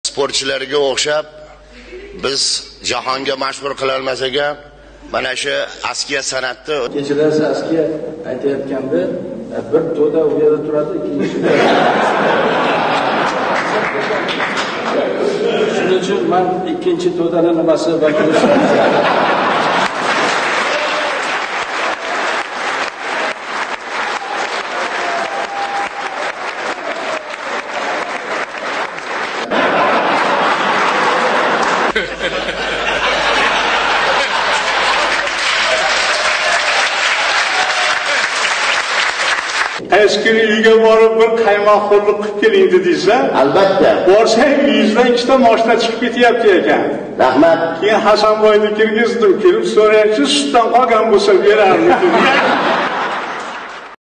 MAMMASIDIQ SHERAEV 77 YOSHIDA OLAMDAN OTTI. ASKIYA SANATI QROLO EDI.🤲
77 yoshda